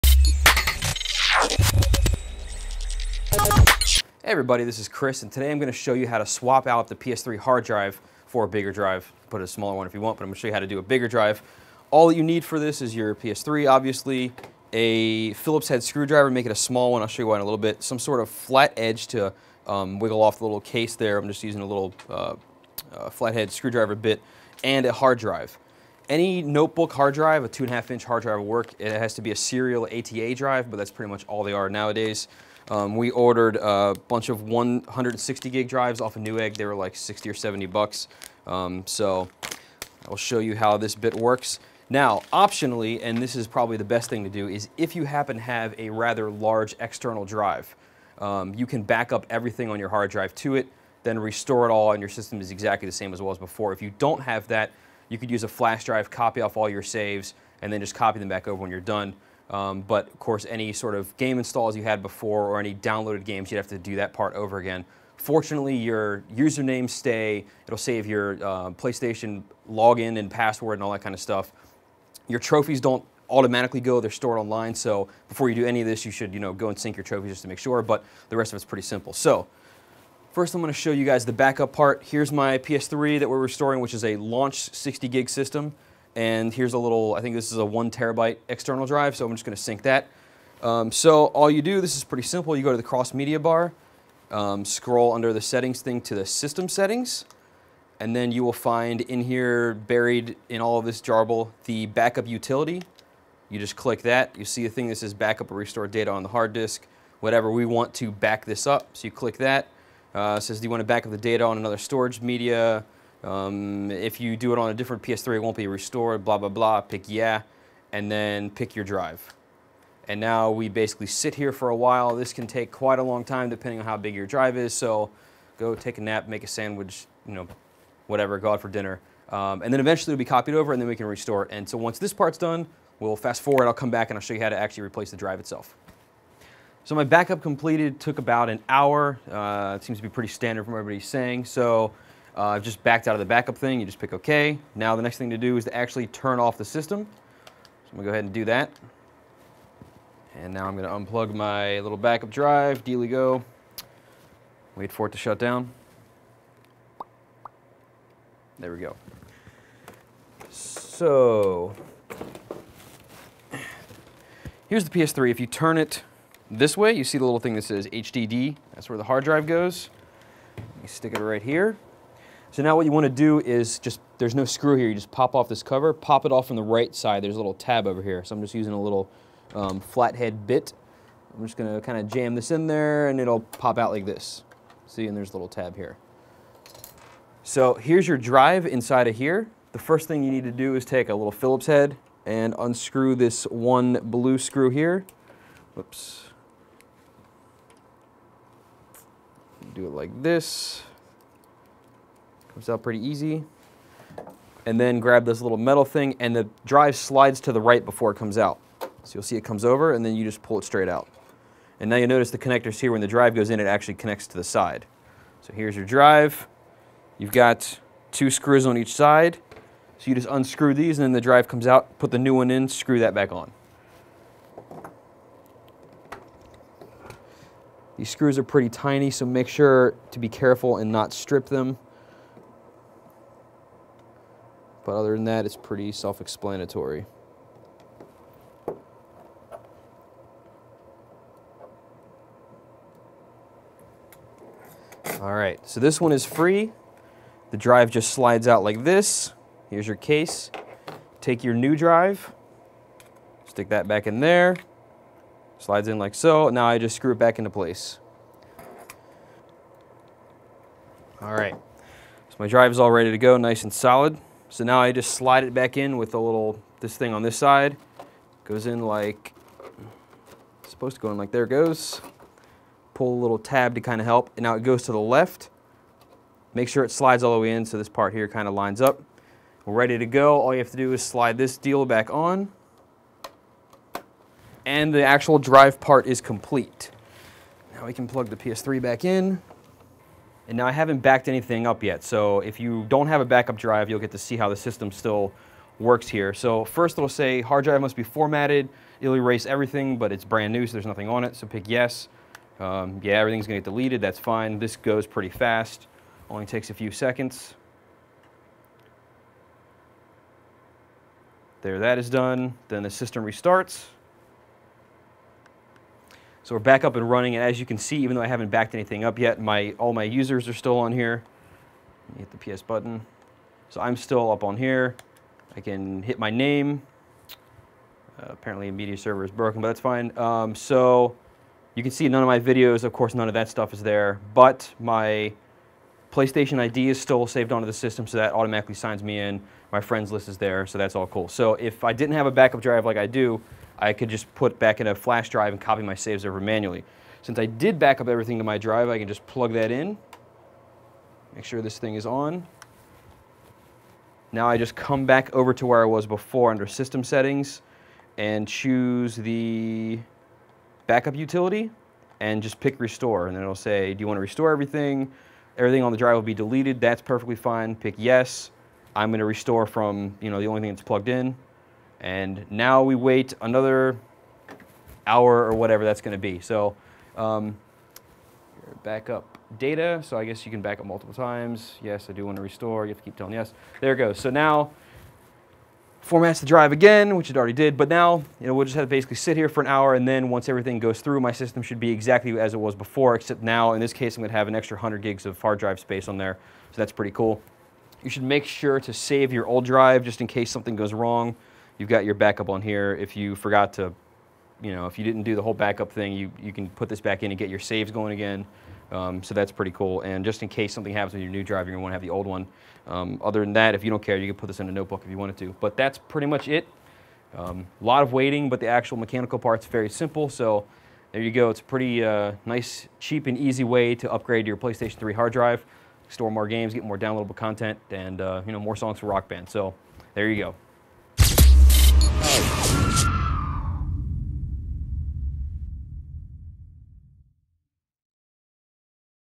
PS3 Hard Drive Upgrade Tutorial
A step by step guide to upgrading the storage space on your PlayStation 3.